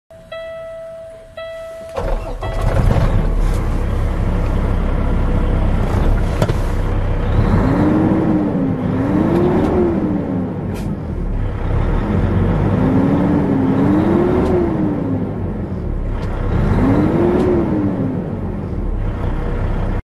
2013 PETERBILT 384 For Sale
2013 PETERBILT 384 DAY CAB TRUCK, Paccar MX Engine, 455 HP, Engine Brake, Eaton 10 Speed Transmission, PTO, 12,000# Front Axle, 38,000# Rears, Twin Screw, 3.70 Ratio, Diff Lock, Pete Air Ride Suspension, Dump Valve, 180" Wheelbase - FILE PHOTO - PICTURES AND ADDITIONAL SPECS WILL BE ADDED WHEN TRUCK ARRIVES AT AUCTION YARD